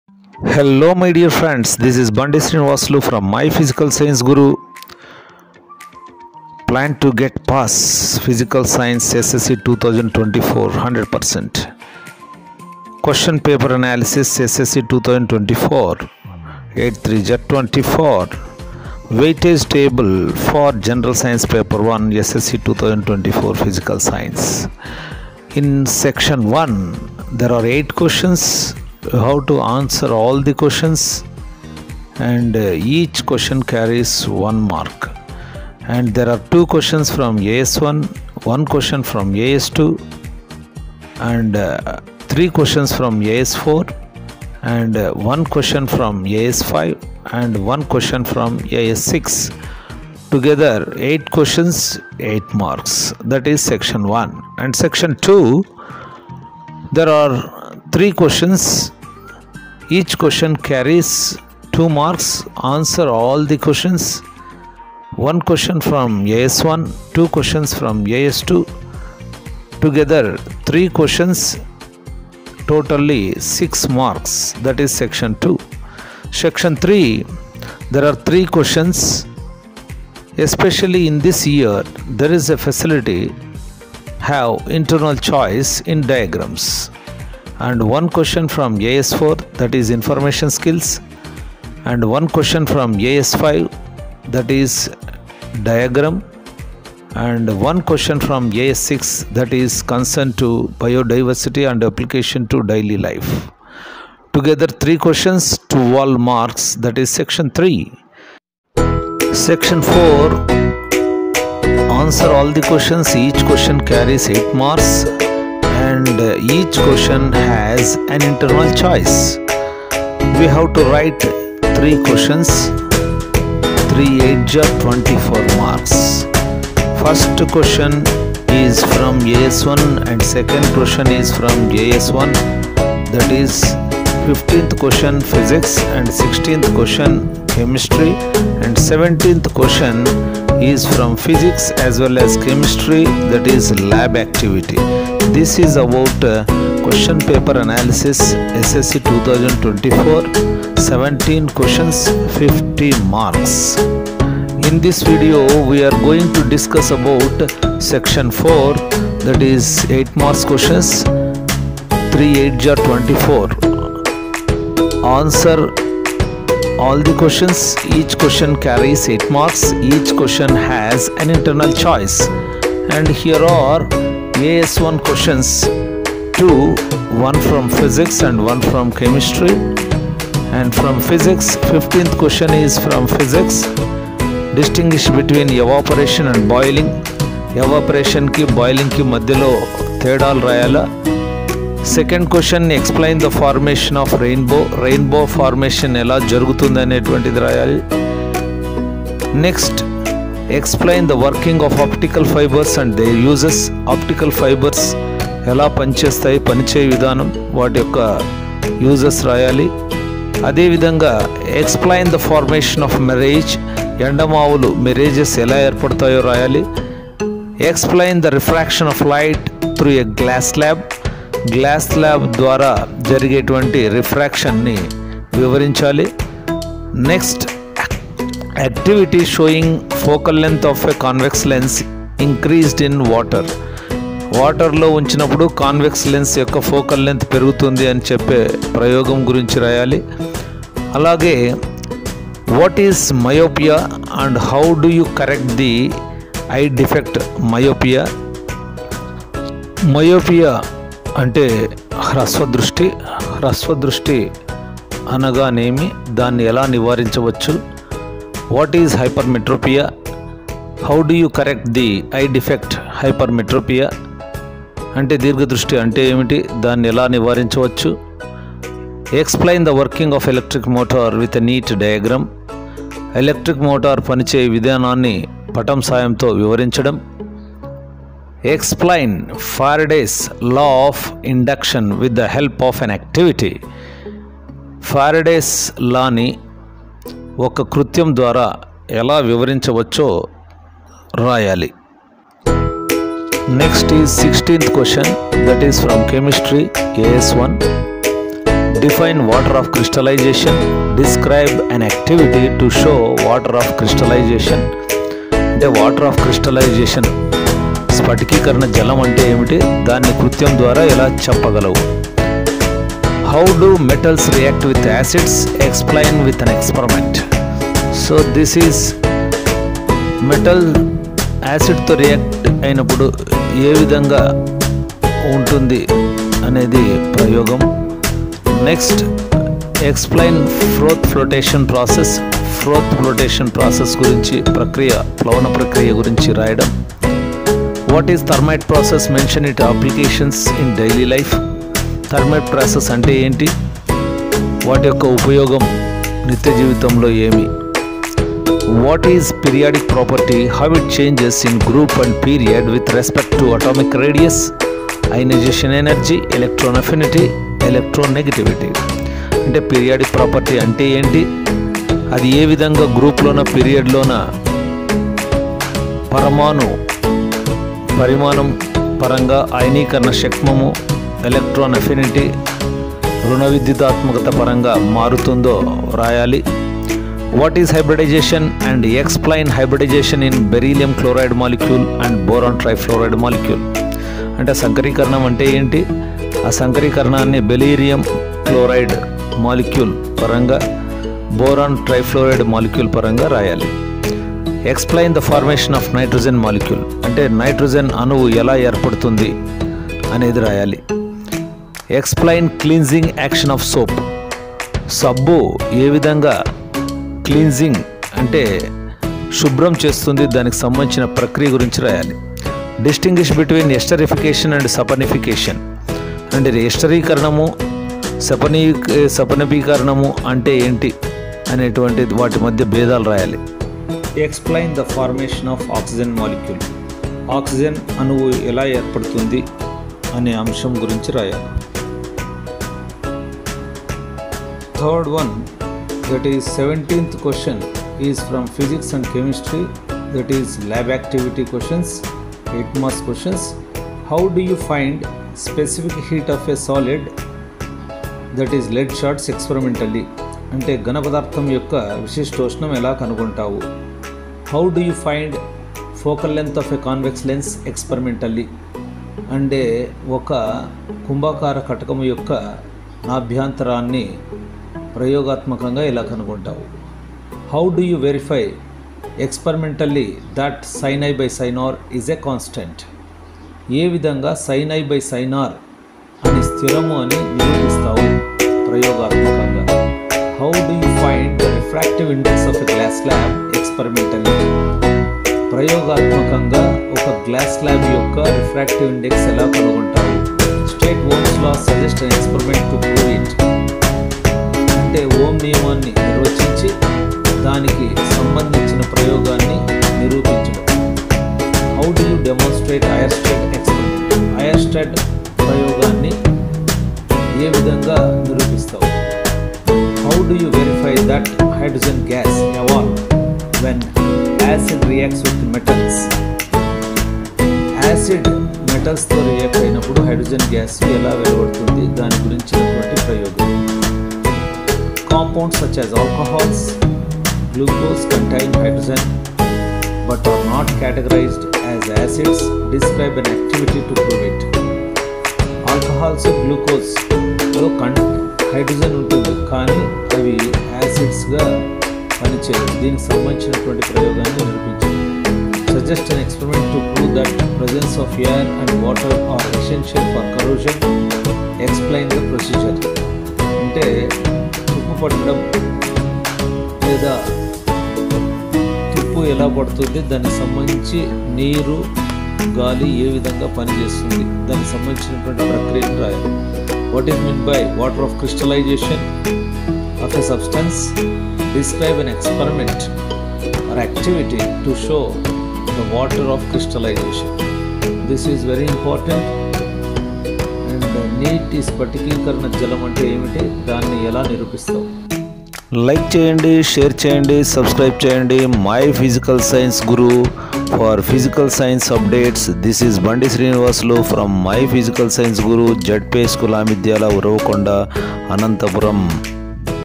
PHYSICAL SCIENCE PAPER ANALYSIS SSC-2024 3×8=24M
15)Q)1.Distinguish between Evaporation and Boiling.
2 Explain the formation of Rainbow?
3 Explain the Working of Optical Fibres and their uses?
4 Explain the formation of Mirage?
5 Explain the Refraction of light through a Glass Slab?
6 Activity showing focal length of convex lens increased in water?
7 What is Myopia? How do you correct the eye defect Myopia?
8 what is Hypermetropia?How do you correct the eye defect Hypermetropia?
9 Explain the working of electric motor with a neat diagram?
10 Explain Faraday's law of induction with the help of an activity?                                 16)Q)1 Define water of crystallization? Describe an activity to show Water of Crystallization?
2 How do metals react with acids? Explain with an experiment?
3 Explain Froth Flotation Process?
4 What is Thermite Process? Mention it's applications in daily life?
5 what is Periodic property? How it changes in Group and Period with respect to A) Atomic Radius B)Ionization Energy C) Electron Affinity D) Electronegativity
6 what is Hybridization? Explain Hybridization in
A)Beryllium Chloride molecule B) Boron Trifluoride Molecule
7 Explain the formation of Nitrogen molecule?
8 Explain Cleansing Action of Soap?
9 Distinguish between Esterification and Saponification?
10 Explain the Formation of Oxygen Molecule?                                                      17)Q)1. How do you find specific heat of a solid (lead shots) experimentally?                                                                                2. How do you find focal length of a convex lens experimentally?
3. How do you verify experimentally that Sin i / Sin r is a constant?
4. How do you find the refractive index of a glass slab experimentally?
5. State Ohm's law. Suggest an experiment to verify it?
6. How do you demonstrate Oersted's experiment?
7. How do you verify that hydrogen gas evolve when acid reacts with metals?
8. compounds such as alcohols and glucose contain hydrogen but are not categorised as acids. Describe an activity to prove it?
9. suggest an experiment to prove that the presence of air and water are essential for corrosion. Explain the procedure?
10. what is meant by water of crystallization of a substance? Describe an activity to show the water of crystallization?